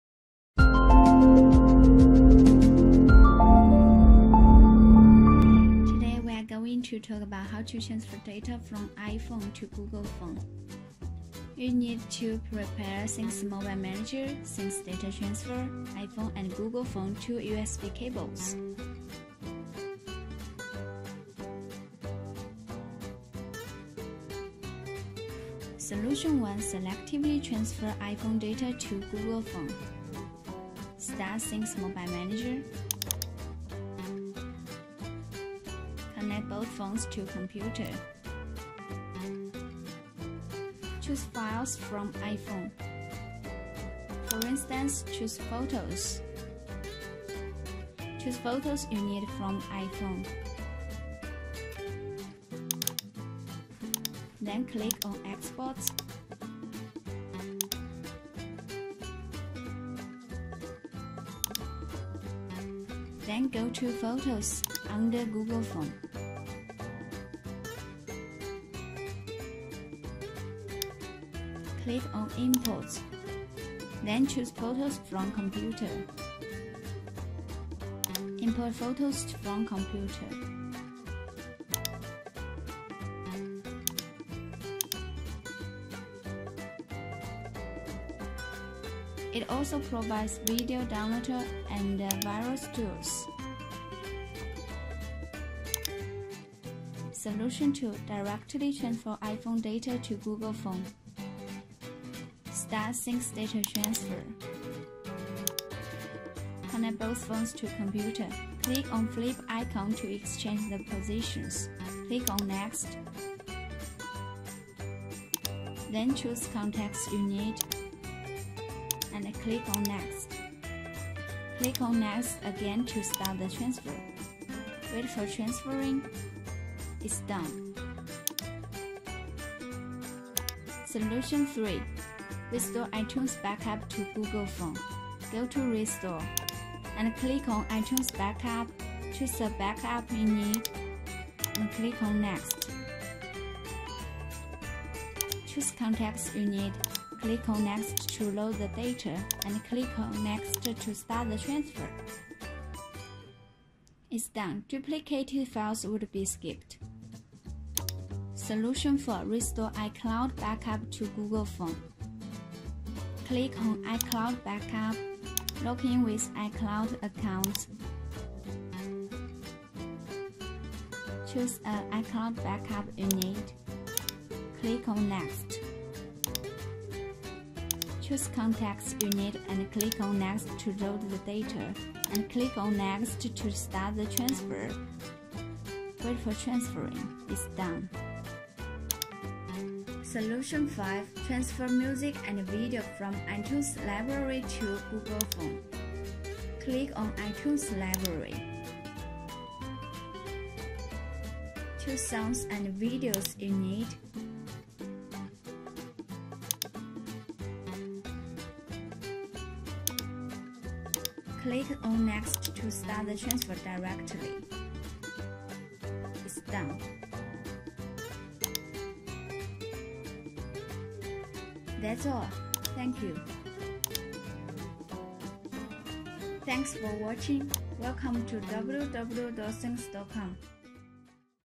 5 Solutions to Sync iPhone Data to Google Pixel 4a
5 effective solutions tell you how to transfer data from iPhone to Google Pixel 4a easily.

Syncios Mobile Manager and Syncios Data Transfer, best data transfer tool which helps you transfer photos, videos, music, contacts, apps and more between devices and also backup and restore phone data in 1-click.

Step 1: Download and Launch Syncios Mobile Manager, connect both phones to computer via USB cables.
Step 2: Choose files from iPhone then click on "Export/Backup" to save files from iPhone to PC.
Step 3:  Then go to Google phone and click on "Add/Import/Recover" to import files from computer to Google phone.

Step 1: Download and Launch Syncios Data Transfer, connect both phones to computer via USB cables.
Step 2: Go to "Transfer" mode on main interface. Click "Next" button.
Step 3:  Then select contents you need, and click on "Next" to load the seleted data and finally click on "Next" to start the transfer.